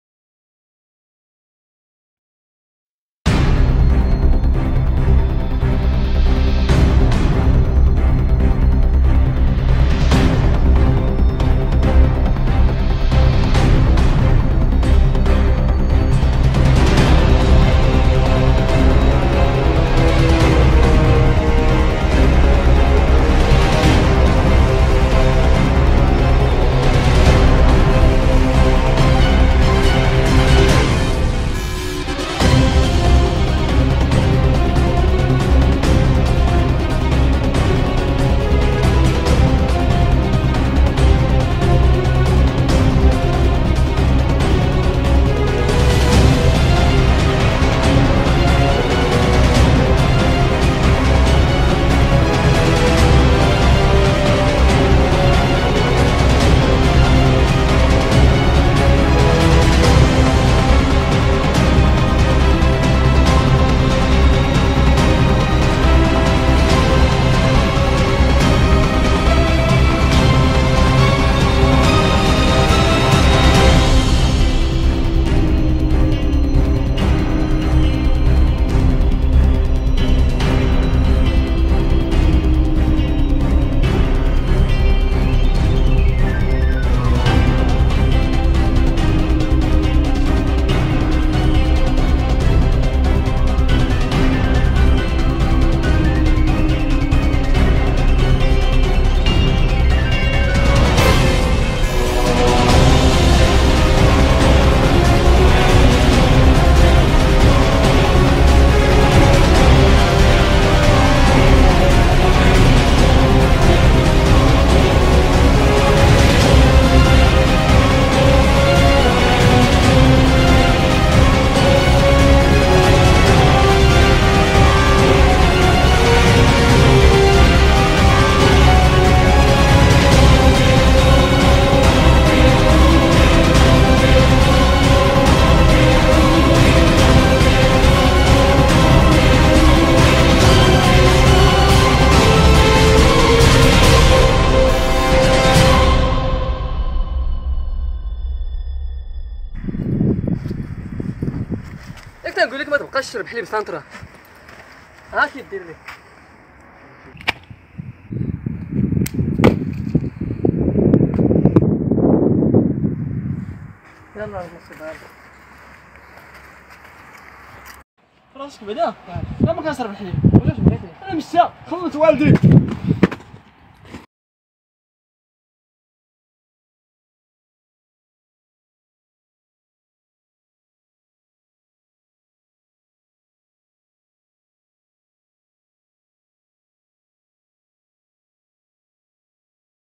Parkour Taroudant - باركور تارودانت
باركور بمشاركة شباب من مدينة تارودانت